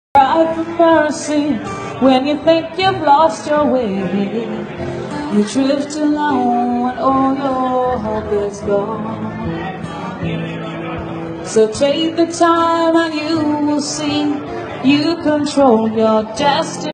Deepsnake Hellsinki at Jyvänen Myllypuro 090218
Deepsnake Hellsinki did East Helsinki in February 2048 (Jyvänen Myllypuro). Marianne's video.